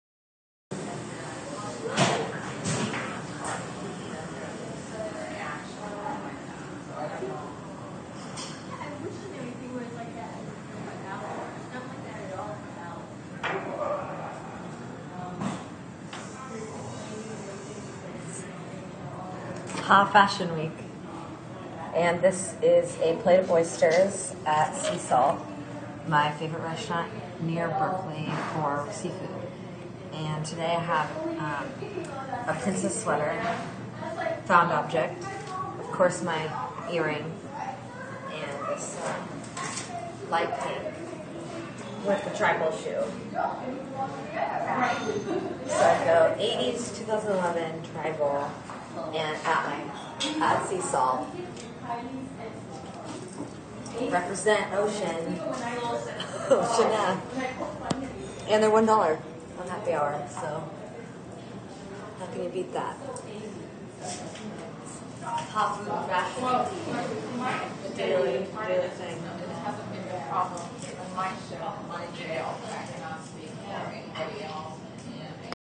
FASHION: Sea Salt oysters in 3-D. Heather Holton in Berkeley on HTC 3D Android.
FASHION: Sea Salt oysters in 3 Dimensions. Heather Holton on HTC 3D Android.

(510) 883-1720Hours: Sun, Sat 10am--10pm; Mon-Fri 11:30am--10pm
Bus: Dwight Way:San Pablo Av